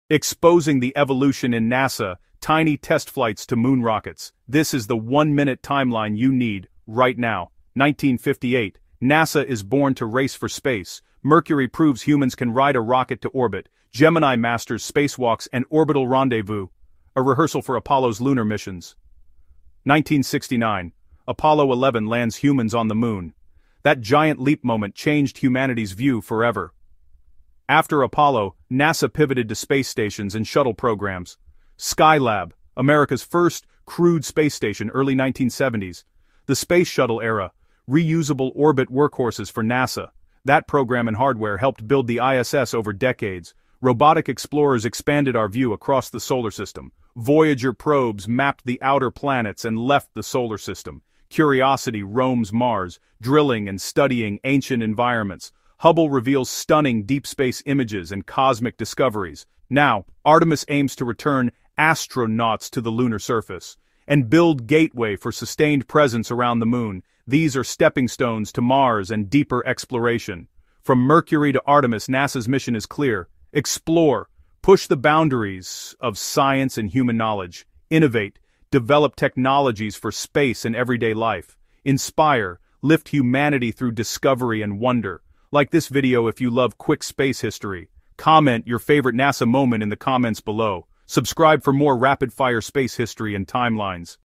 The History of NASA: From Apollo to Artemis | NASA Space Exploration Timeline Explained
Explore the incredible journey of NASA — from the historic Apollo Moon landings to the modern Artemis missions leading humanity back to the Moon and beyond. 🌕🚀

This video covers NASA’s evolution, key missions, and the future of space exploration that continues to inspire generations.